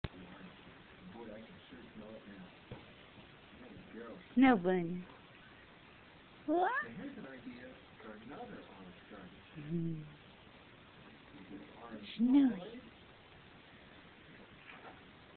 Snowy at rest
Snowy a/k/a Snowbunny sleeping in the sun, well, trying to sleep but his Meowmie keeps calling his name - hard to believe he was once a wild stray with no intentions of ever being a gentleman house cat :)